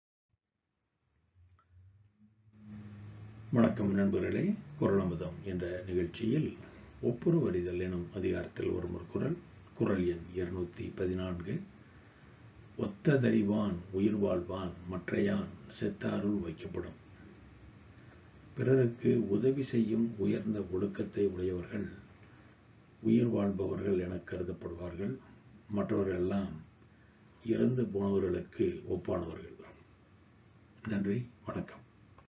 Thirukural 214 அறத்துப்பால்: இல்லறவியல்: அதிகாரம்: ஒப்புரவறிதல்
அறத்துப்பால்: இல்லறவியல்: அதிகாரம்: ஒப்புரவறிதல்: குறள் எண்: 214
ஒத்த தறிவான் உயிர்வாழ்வான் மற்றையான்
செத்தாருள் வைக்கப் படும்
பிறருக்கு உதவி செய்யும் உயர்ந்த ஒழுக்கத்தையுடையவர்கள் உயிர் வாழ்பவர்கள் எனக் கருதப்படுவார்கள். மற்றவர்களெல்லாம் இறந்து போனவர்களுக்கு ஒப்பானவர்கள்.
-உரை : .ந.ப.